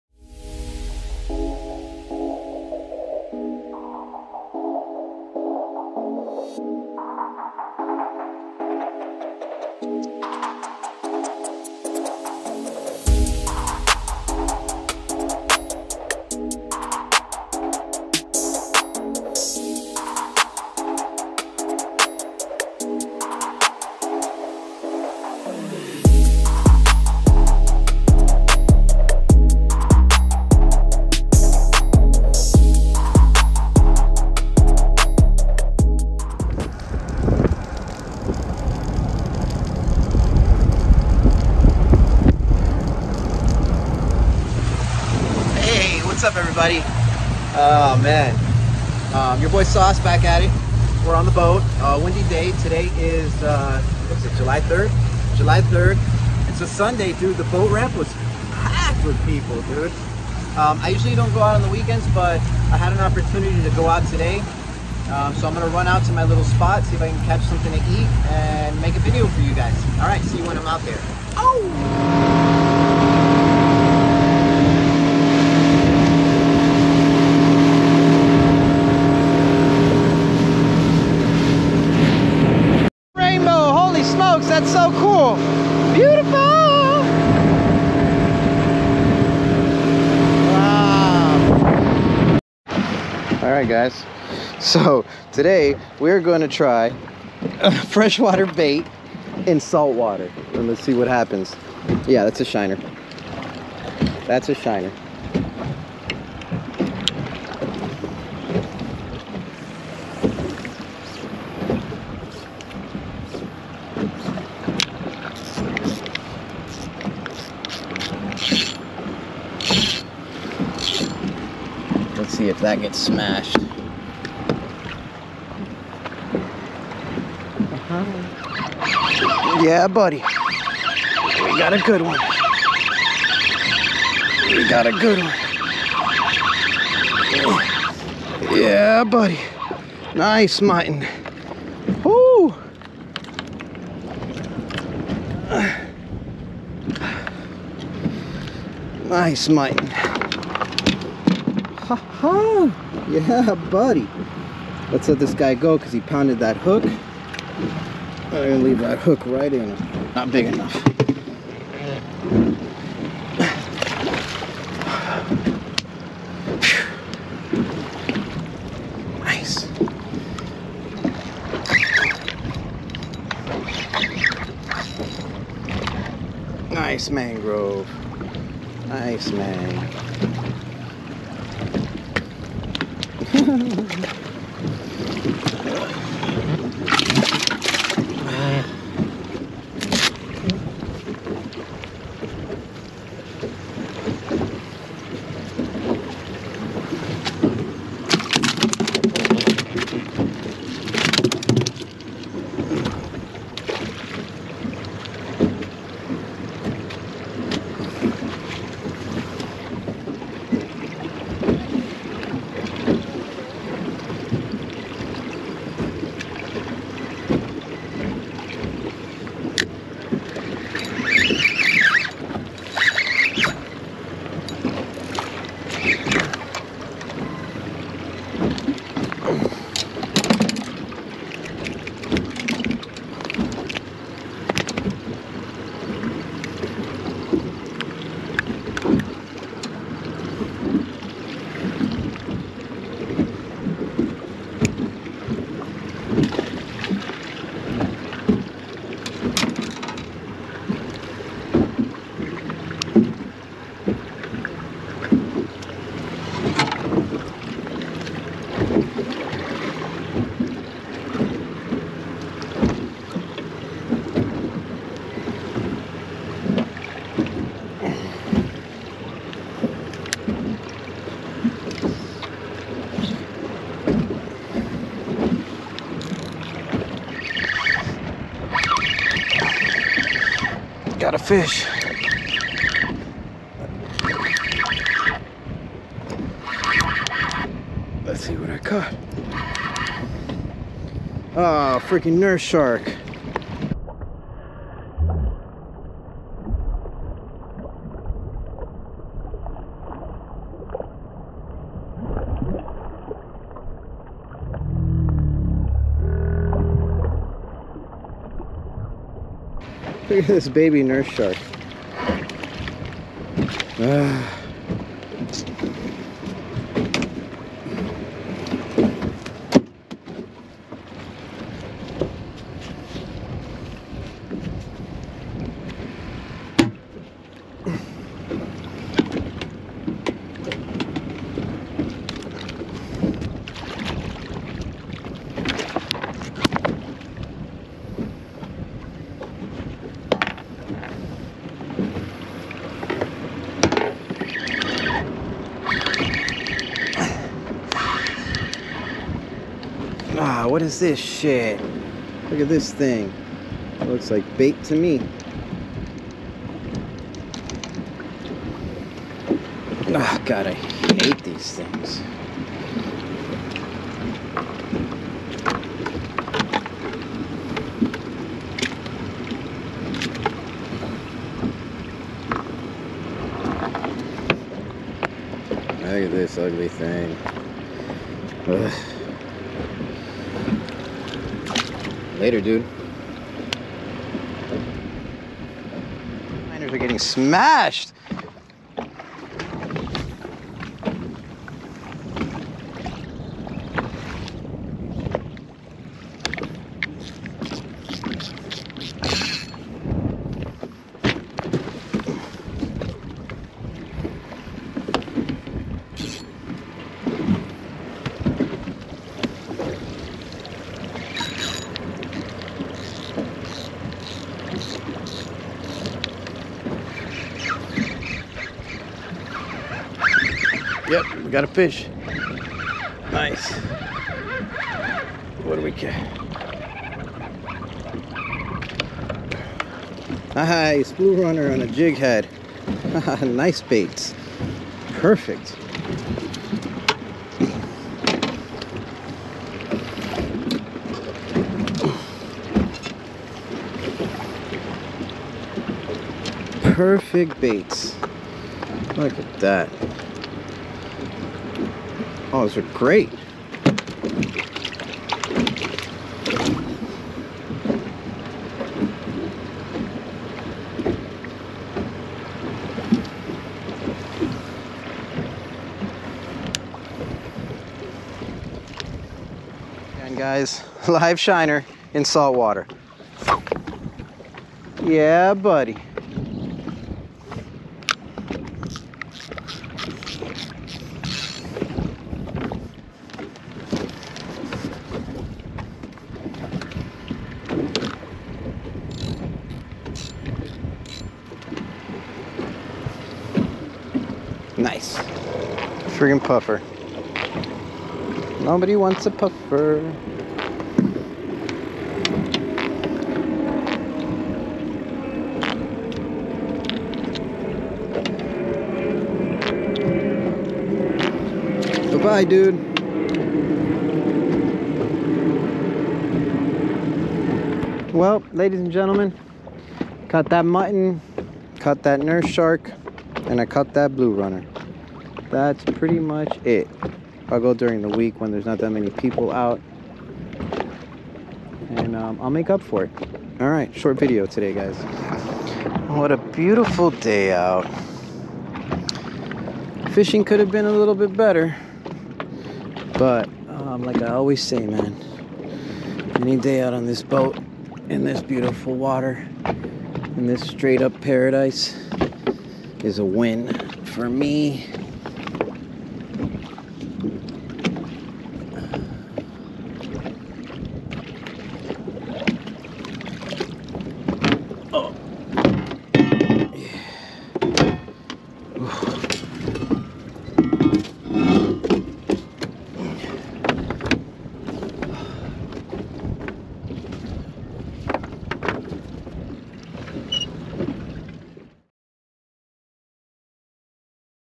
FISHING MUTTON SNAPPERS IN KEY BISCAYNE FLORIDA
Took a quick trip to Biscayne Channel out by the tripod for some snapper fishing. I will definitely Never fish a holiday weekend again. Had to move around due to rude boaters anchoring right next to me during this July 4th weekend. Still able to hook up to some nice fish! Watch and enjoy and like always be safe out there.